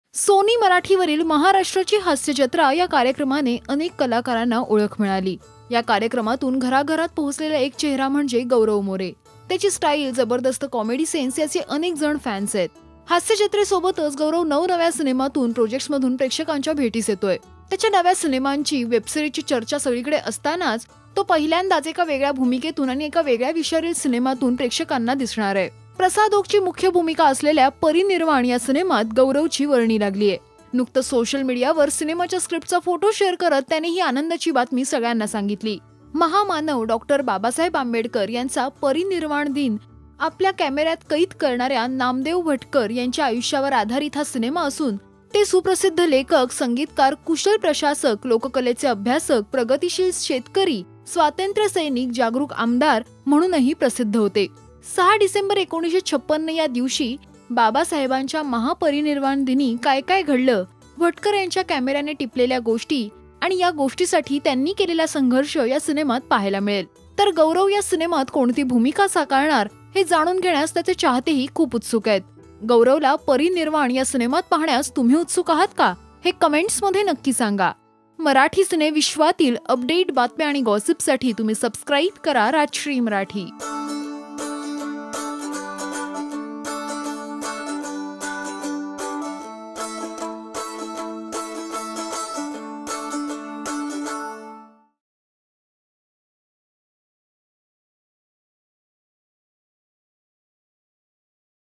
गौरव साकारणार गंभीर भूमिका | Gaurav More's New Movie | Maharashtrachi Hasya Jatra
अभिनेता गौरव मोरे नव्या सिनेमातून आणि नव्या भूमिकेतून प्रेक्षकांच्या भेटीस येतोय. जाणून घेऊया याविषयी या व्हिडिओमध्ये.